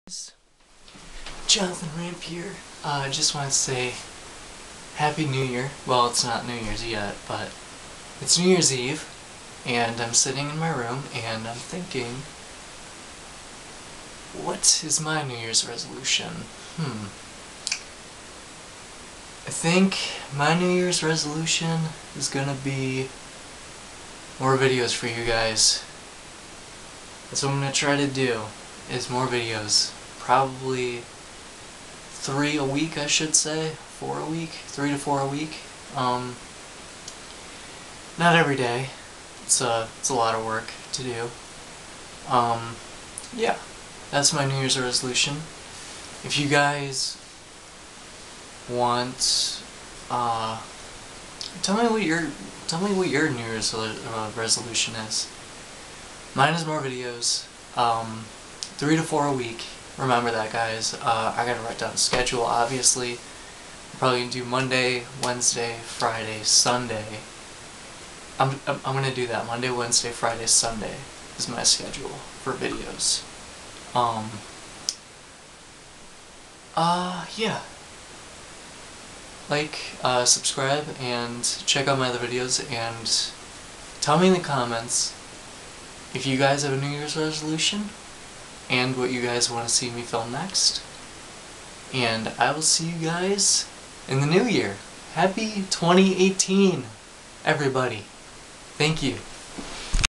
New Years Resolution!
I do a video wishing you guys a happy new year for 2018.